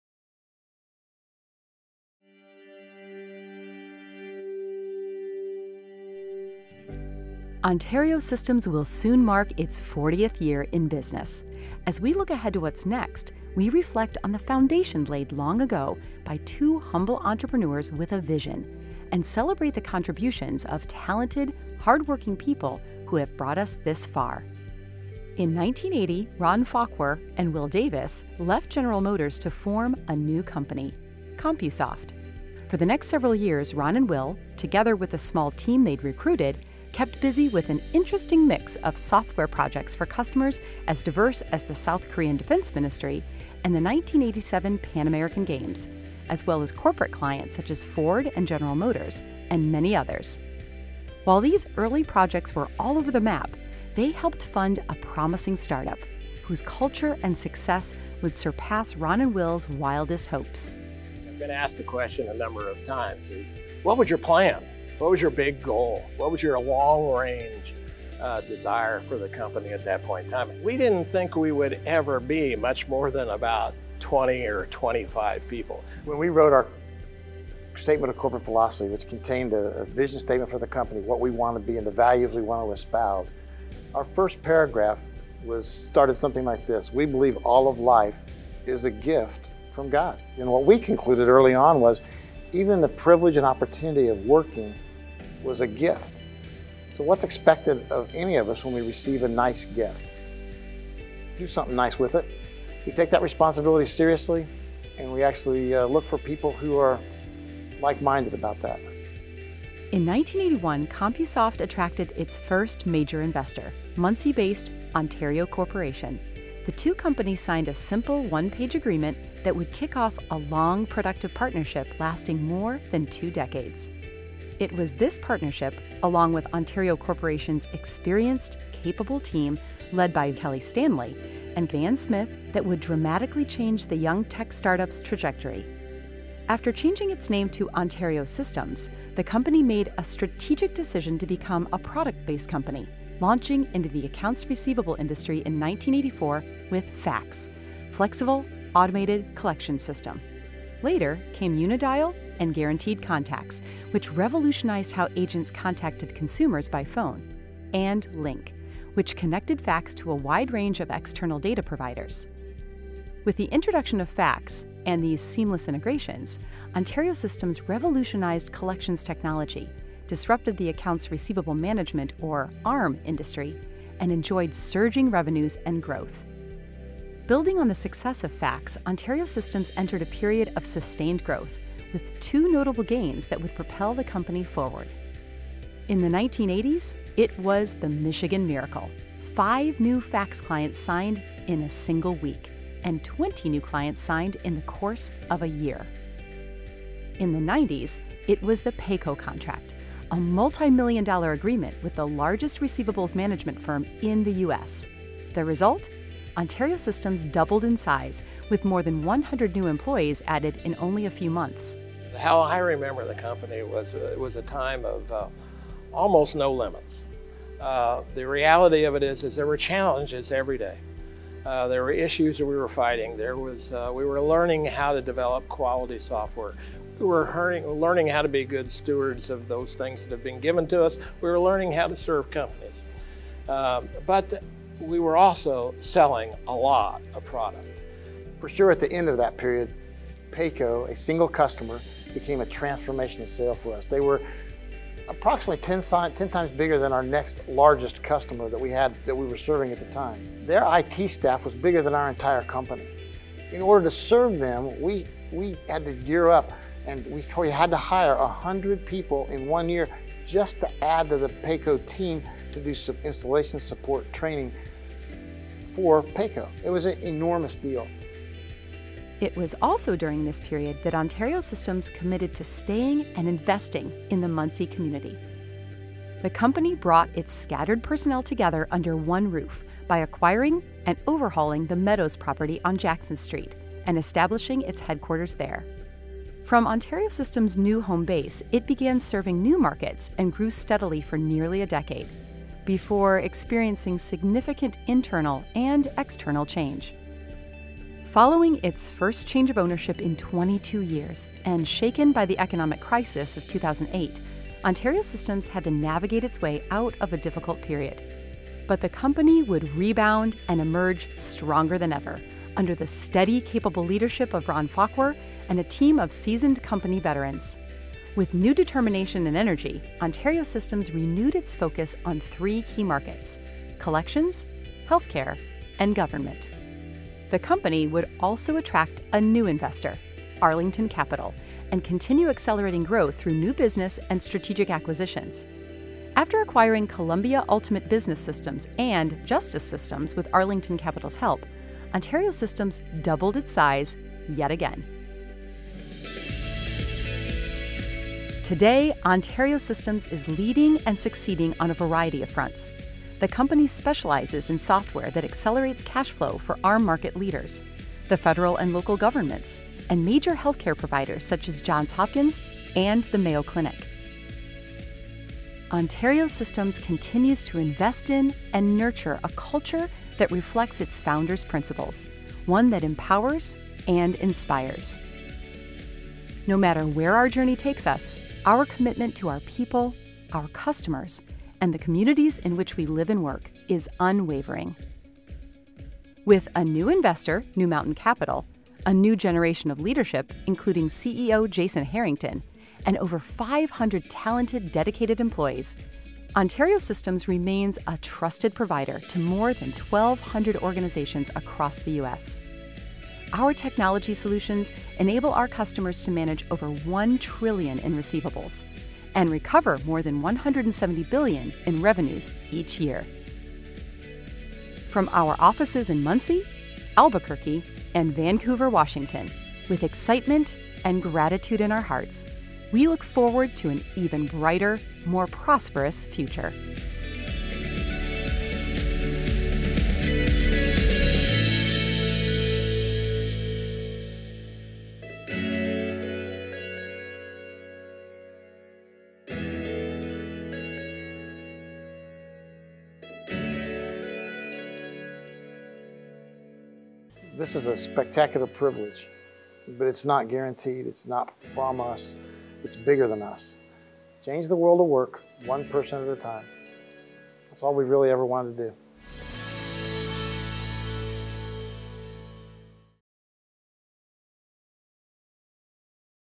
Ontario Systems -- Celebrating 40 Years
Ontario Systems hosted a community event at its Muncie headquarters Thursday, November 14th to celebrate the company’s nearly 40-year history and announce a new community fund to help improve quality of life for area residents.

This video recounts the company’s humble beginnings, milestones, and the future.

Members of Ontario Systems’ current company leadership, current and past employees, and members of the Muncie community gathered for the two-hour-long event to reminisce and reflect on Ontario Systems’ history, the important contributions of cofounders Wil Davis and Ron Fauquher, the company’s growth and success, and its close relationship with and commitment to the Muncie community.